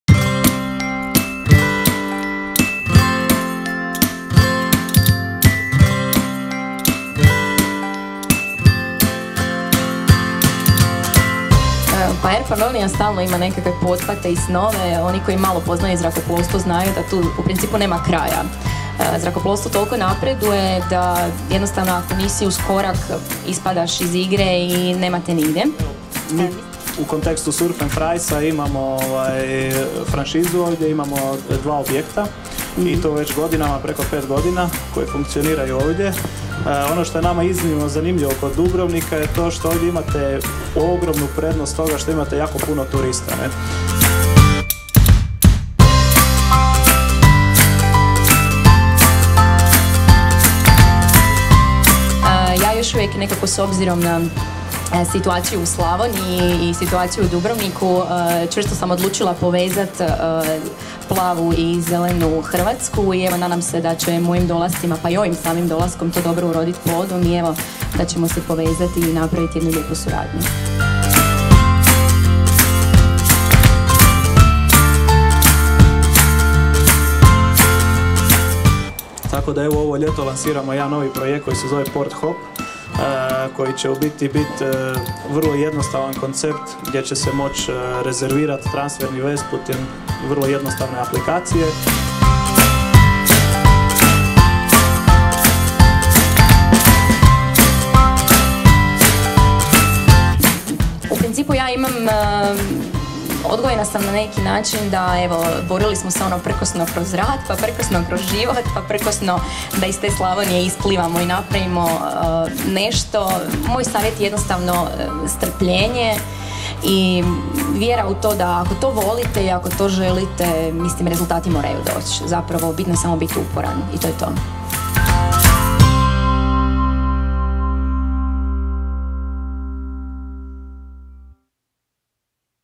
7. Business Café Dubrovnik - reportaža 2 min
7. Business Café Dubrovnik održan je 16. svibnja u 19 sati u Restoranu Culto u Dubrovniku.

Tema je bila – Samo nebo je granica – a gosti Zrinka Vlašić, direktorica Air Panoniae, privatne zrakoplovne tvrtke sa sjedištem u Osijeku i Andrija Čolak, serijski poduzetnik, vlasnik poznate franšize Surf’n’Fries i suosnivač startupa Kisha.

U kratkoj reportaži pogledajte što smo novo naučili.

Kako je bilo na prethodnom Business Caféu Dubrovnik možete vidjeti na sljedećim linkovima: